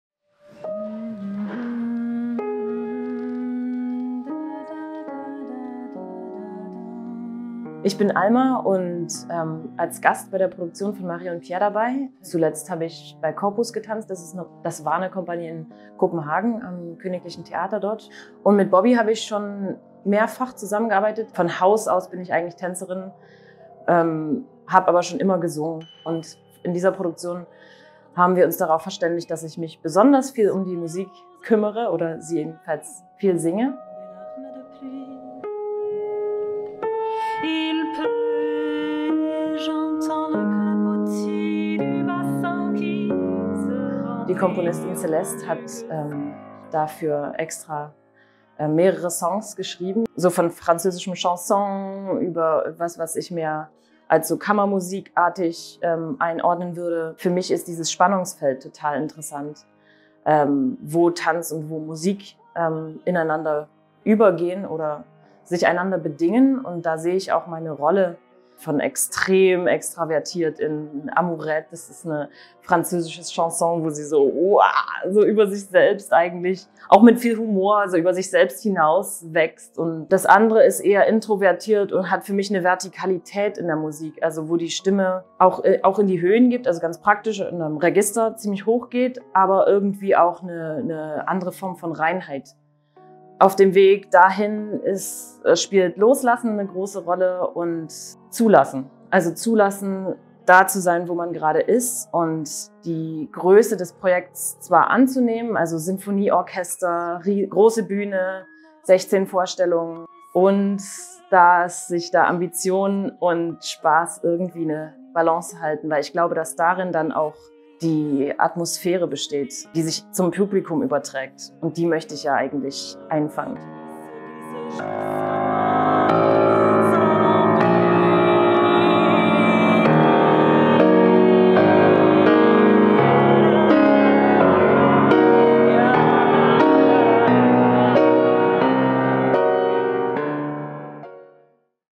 ‹Marie & Pierre› – Tänzerin und Sängerin Alma Toaspern über das Stück
‹Marie & Pierre›

Choreographie – Bobbi Jene Smith
Musikalische Leitung – Tianyi Lu, Thomas Herzog
Auftragskomposition – Celeste Oram
Bühne – Christian Friedländer
Kostüme – Christian Friedländer, Bobbi Jene Smith
Lichtdesign – Roland Edrich
Dramaturgie – Judith Vrancken, Jonathan Earl Fredrickson

Cellistin – Valentina Dubrovina, Violinist / Pianist – Keir GoGwilt, Trompete – Immanuel Richter, Morgan Huw
Sinfonieorchester Basel

Es tanzt das Ballett Basel

Video: Theater Basel